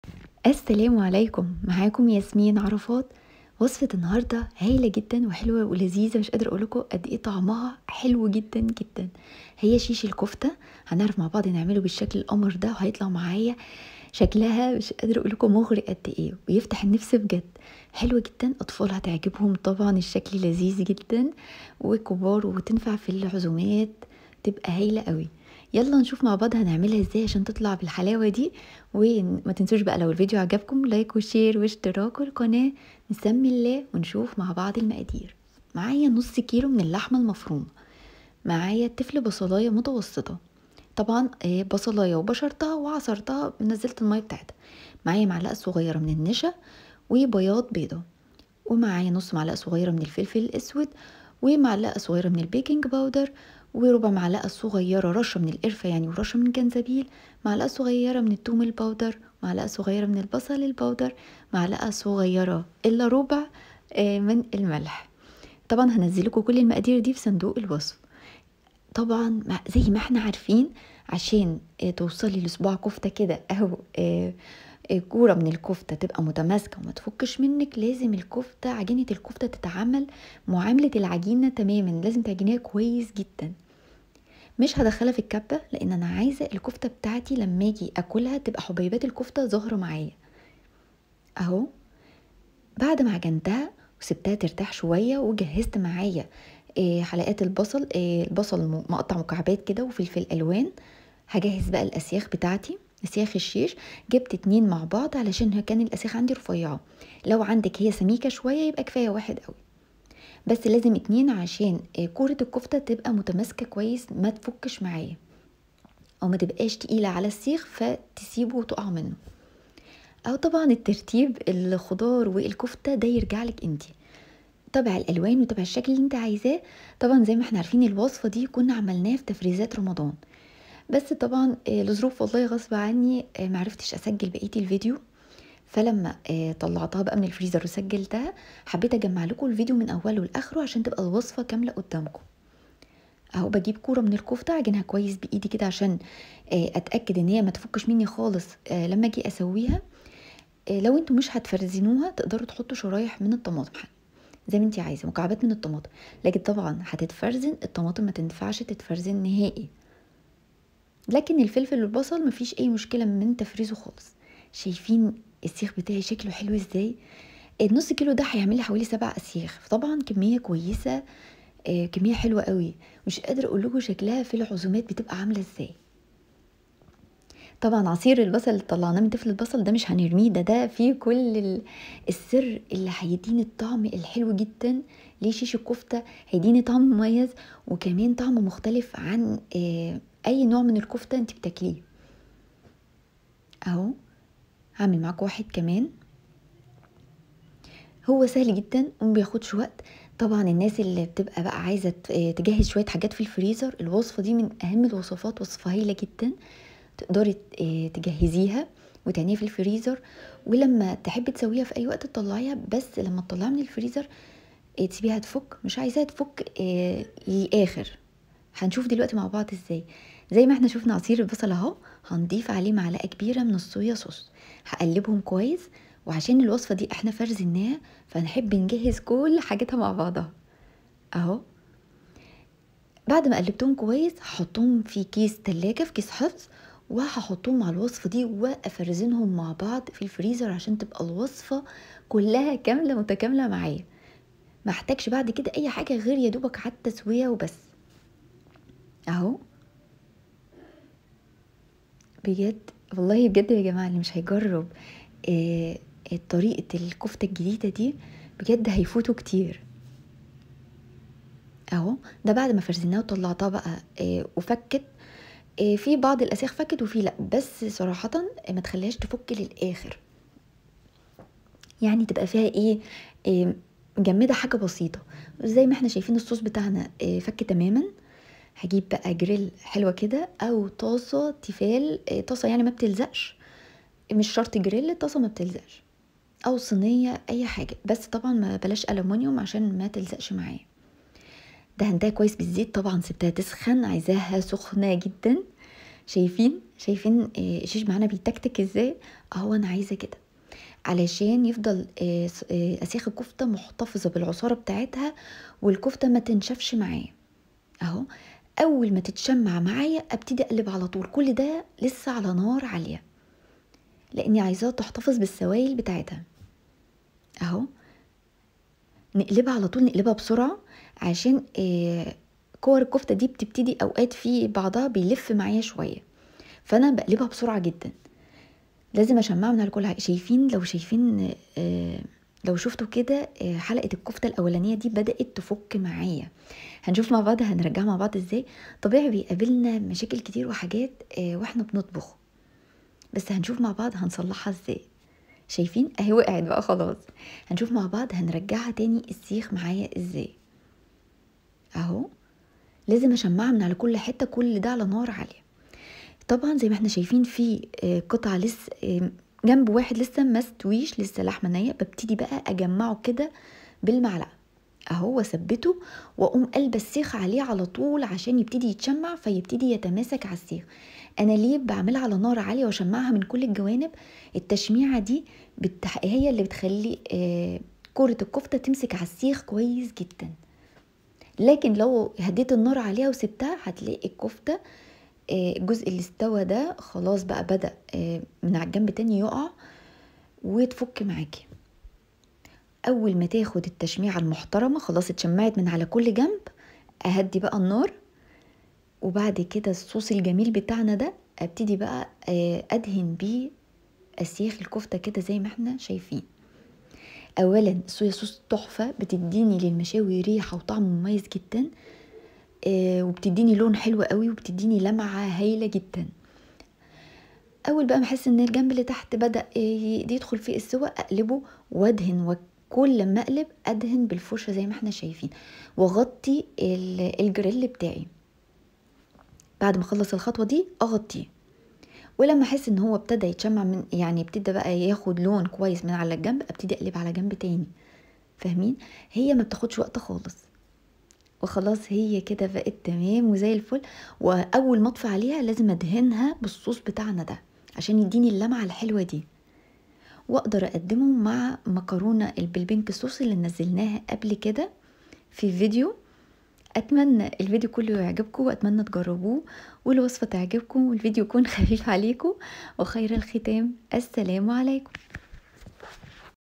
شيش الكفتة اللذيذ😋 بطريقة سهلة وبسيطة والطعم ولااروع😋😋
هتعرفى تعملى الكفتة بطريقة مختلفة والطعم لذيذ جدا

وهتعملى احلى شيش كفتة وتنفع جدا ف العزومات

مقادير شيش الكفتة